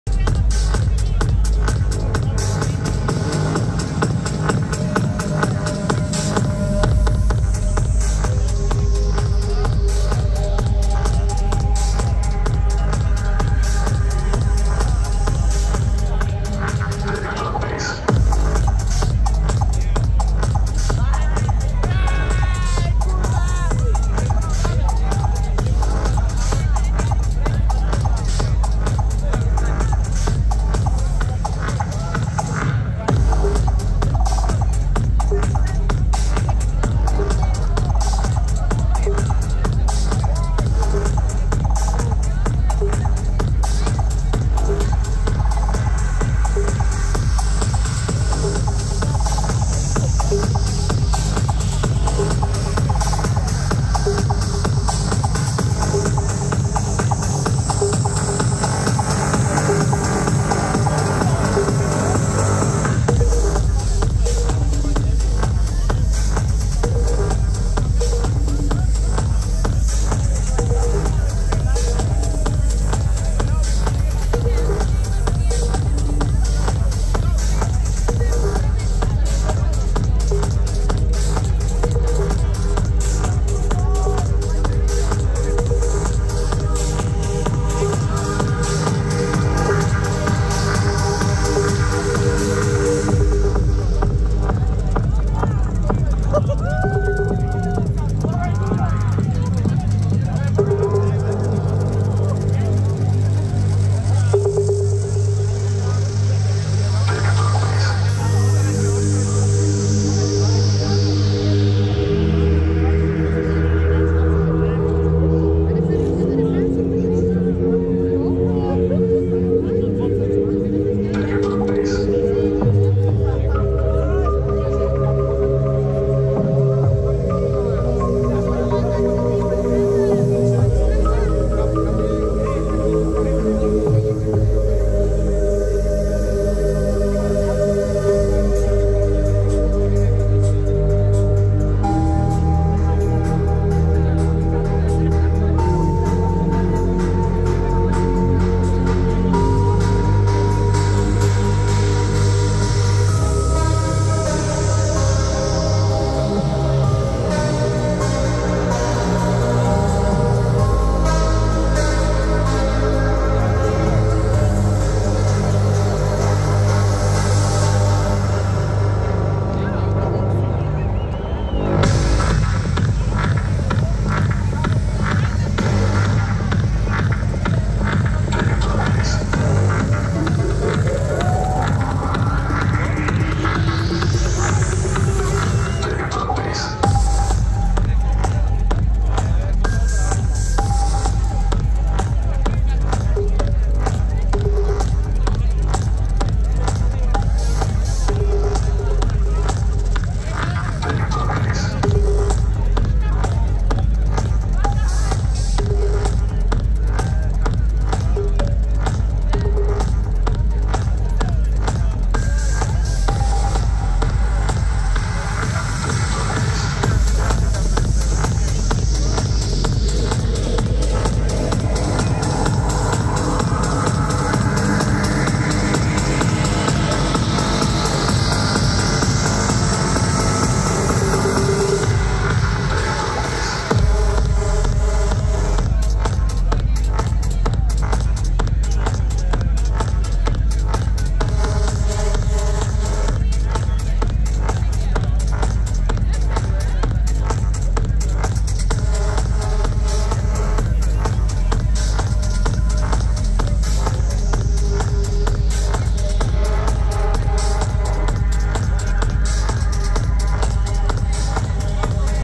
Beats for Love 2024 - Boris Brejcha 2/2
Beats for Love 2024 festival in Ostrava, Czech republic

Love Stage (4.7.2024)

Artist:  Boris Brejcha